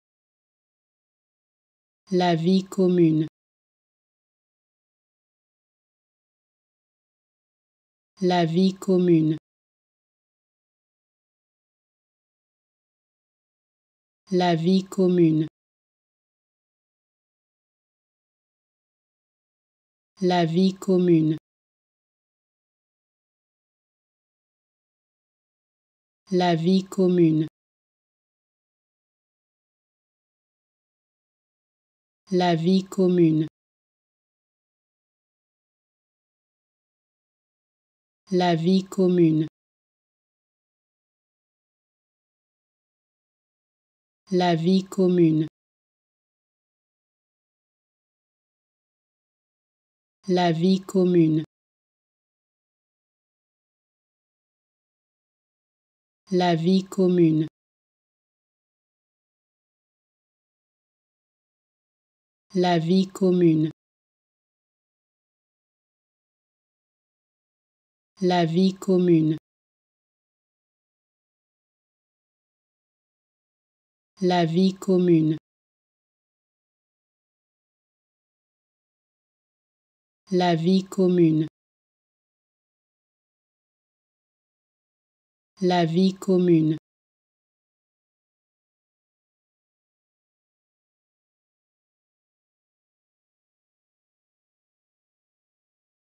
How to Pronounce ''La vie commune'' (Common life) Correctly in French
Learn how to say and properly pronounce ''La vie commune'' in French with this free pronunciation tutorial.